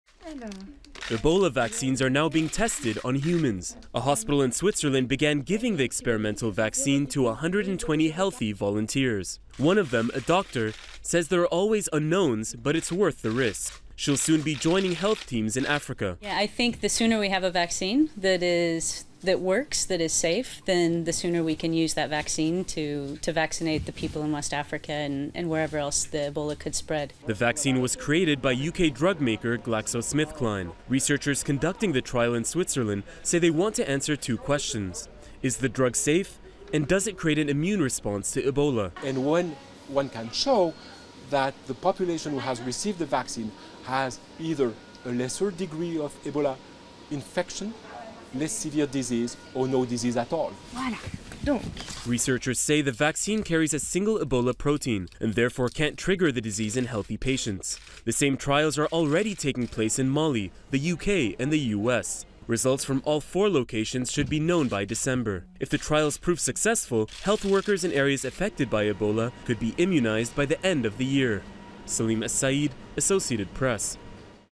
Medical Volunteers Receive Trial Ebola Vaccine
A hospital in Switzerland on Tuesday began testing an experimental Ebola vaccine developed by the UK drugmaker GlaxoSmithKline. They're using 120 healthy volunteers to see if the vaccine creates an immune response to the deadly virus. (Nov. 4)